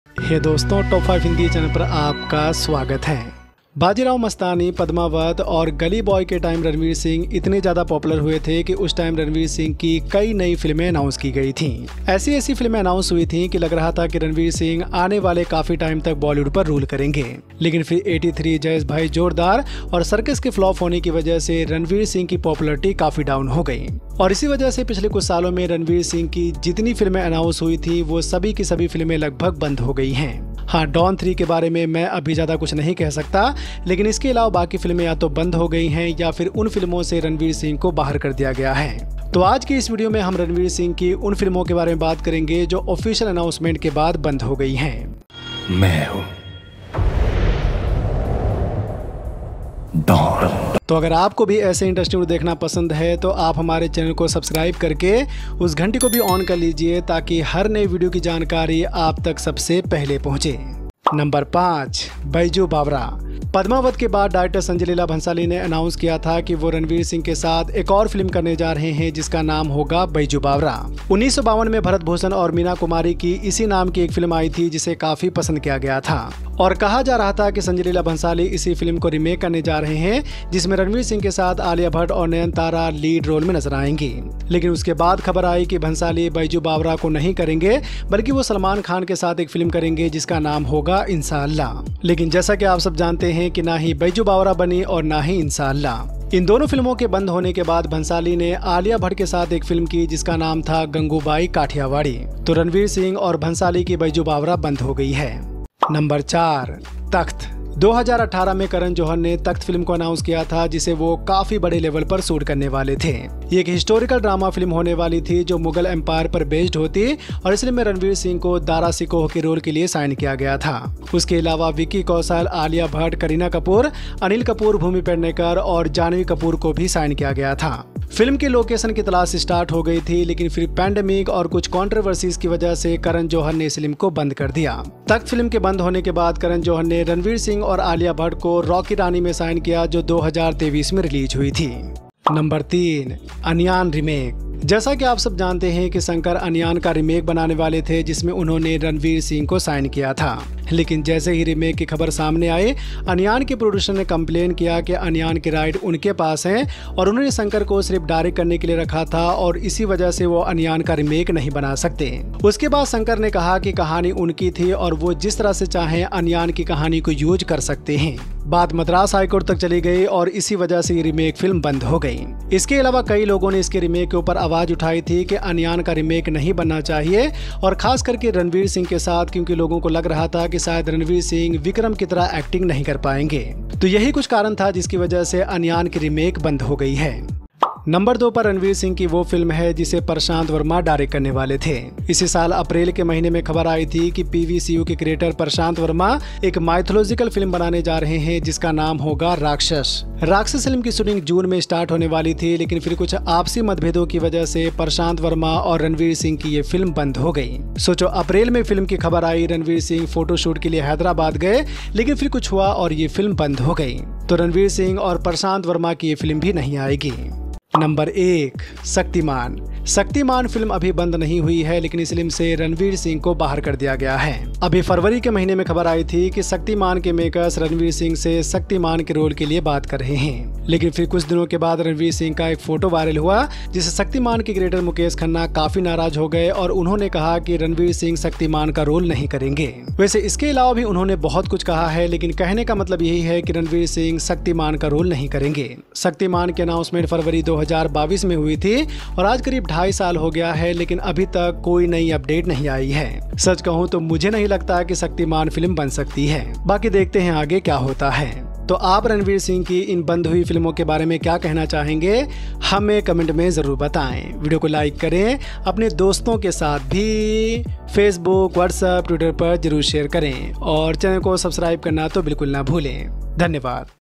Top 5 Ranveer Singh Films Are Shelved After Announcement || रणवीर सिंह की ये फिल्मे बंद हो गयी हैं..
Hey Dosto Swagat Hai Aap Ka 'Top 5 Hindi' Channel Per:
Aaj Hum Baat Karne Wale Hain Ranveer Ki Un Filmo Ke Bare Jo Announcement Ke Bad Band Ho Gayi...